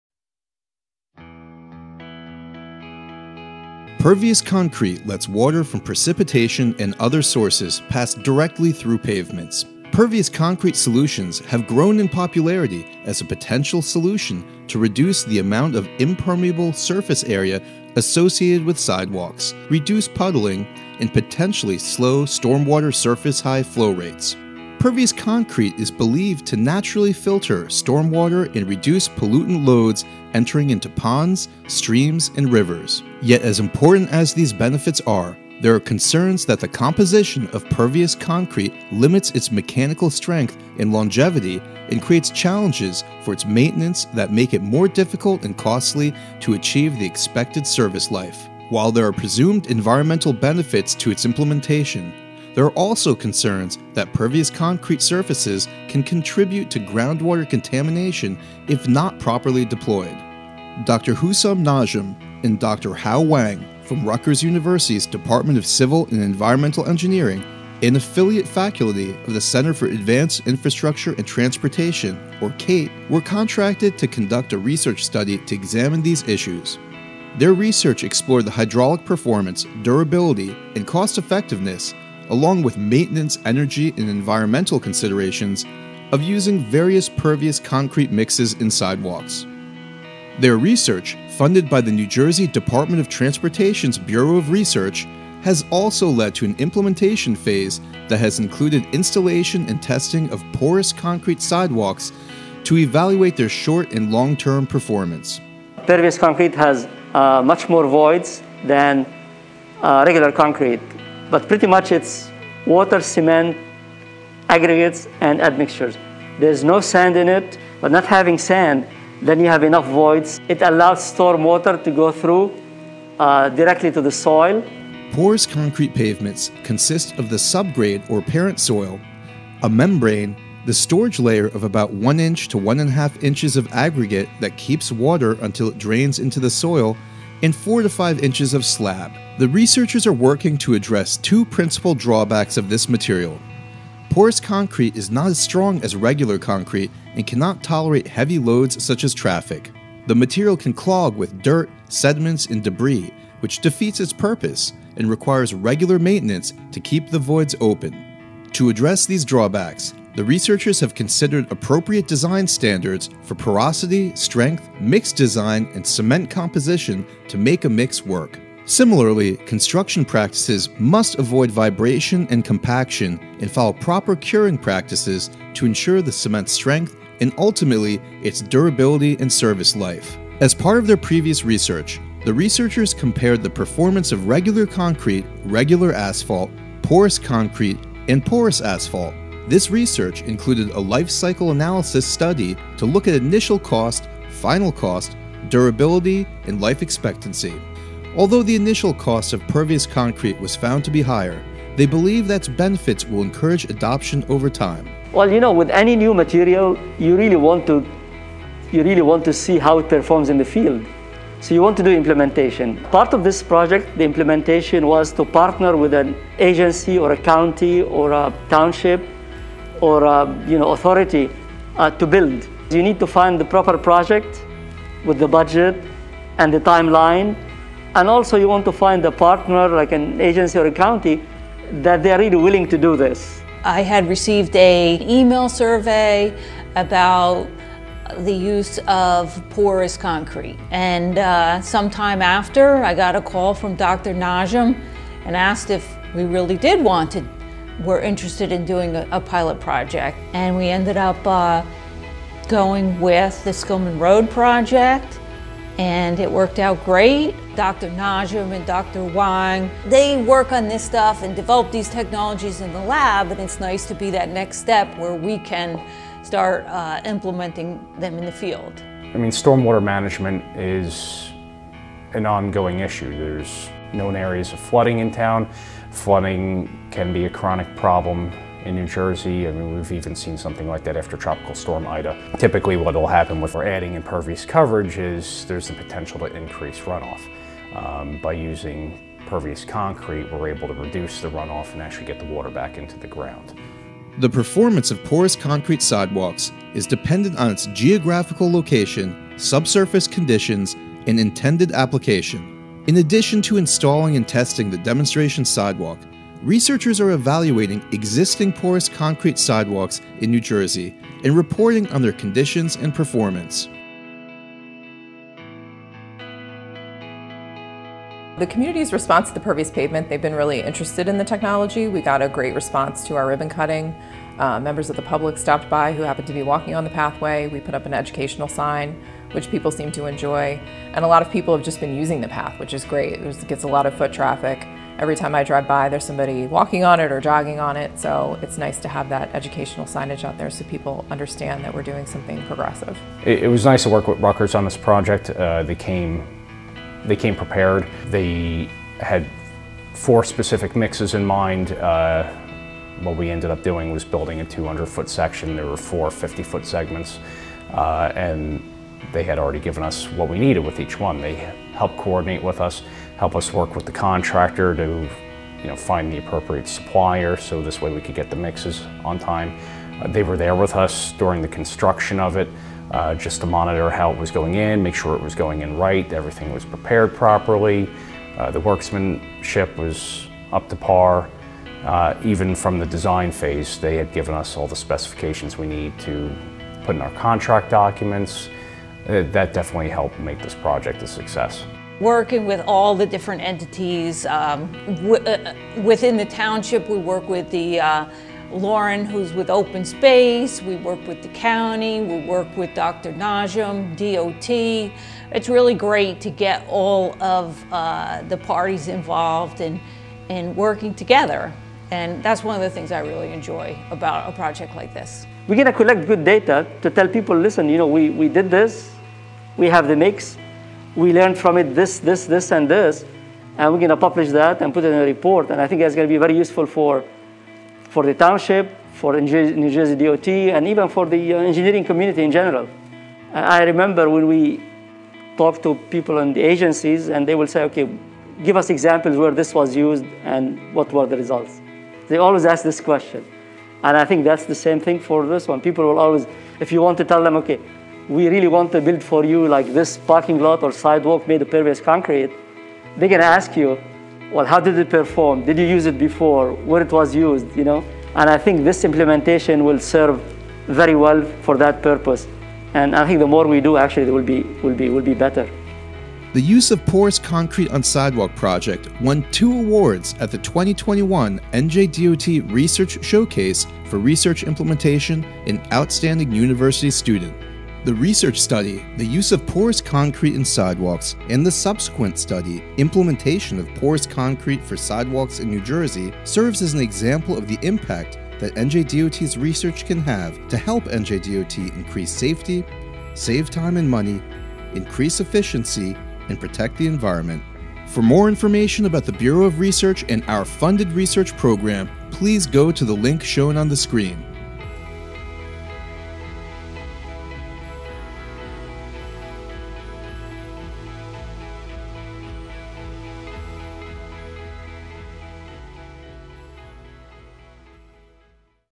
The Use of Porous Concrete in Sidewalks
Short educational and promotional video about the research methods, findings, and implementation steps resulting from NJDOT Bureau of Research sponsored research on the use of permeable (or pervious) concrete for sidewalks.